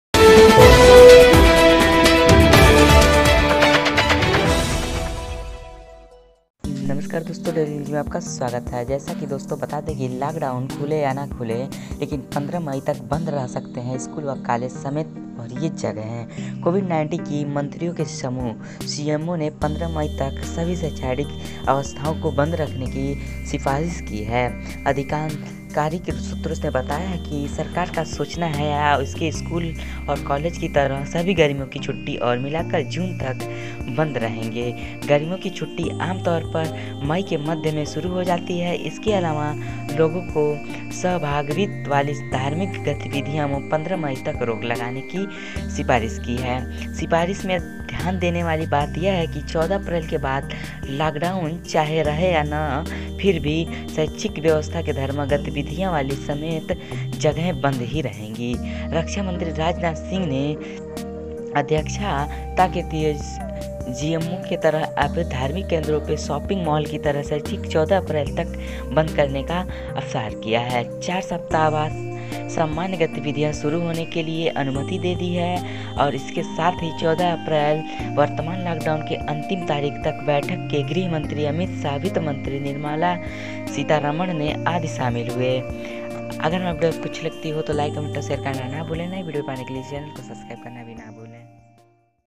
लॉकडाउन चाहे खुले या नहीं, 15 मई तक बंद रह सकती हैं स्कूल कॉलेज समेत ये जगहें
मंत्रियों के समूह (जीओएम) ने 15 मई तक सभी शैक्षणिक संस्थाओं को बंद रखने की सिफारिश की है। आधिकारिक सूत्रों ने बताया कि सरकार का सोचना है कि इससे स्कूलों और कॉलेजों एक तरह से गर्मियों की छुट्टियों को मिलाकर जून के अंत तक बंद रहेंगे। गर्मियों की छुट्टी आम तौर पर मई के मध्य से शुरू हो जाती है। इसके अलावा लोगों की सहभागिता वाली सभी धार्मिक गतिविधियों पर भी 15 मई तक रोक लगाने की सिफारिश की गई है। सिफारिश में ध्यान देने वाली बात यह है कि 14 अप्रैल के बाद लॉकडाउन चाहे रहे या फिर खत्म हो जाए, शैक्षणिक संस्थान व धार्मिक गतिविधियों वाली जगहें बंद ही रहेंगी।

रक्षा मंत्री राजनाथ सिंह की अध्यक्षता वाले जीओएम ने यह तय किया कि धार्मिक केंद्रों, शापिंग मॉल और शैक्षणिक संस्थानों को 14 अप्रैल के बाद कम से कम चार सप्ताह तक सामान्य गतिविधि शुरू करने की अनुमति नहीं दी जा सकती है। 14 अप्रैल वर्तमान लॉकडाउन की अंतिम तारीख है। इस बैठक में गृह मंत्री अमित शाह, वित्त मंत्री निर्मला सीतारमण आदि शामिल हुए।